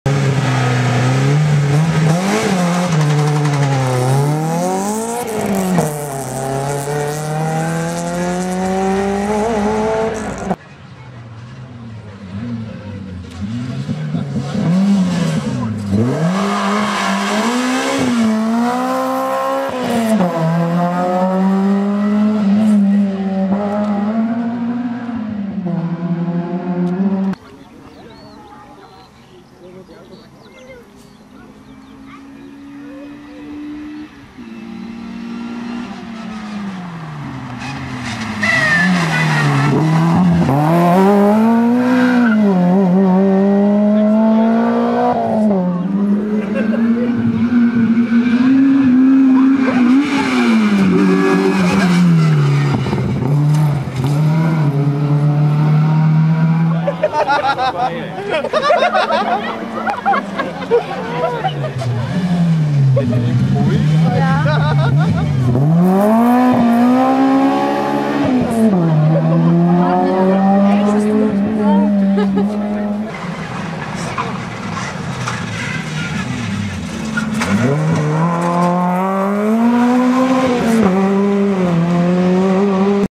13.Fontane Rallye 2012 - Joachim / Becher Startnummer 36
36 Uwe Joachim Wittenberg Sascha Becher Langenbach H 12 Suzuki Swift GTI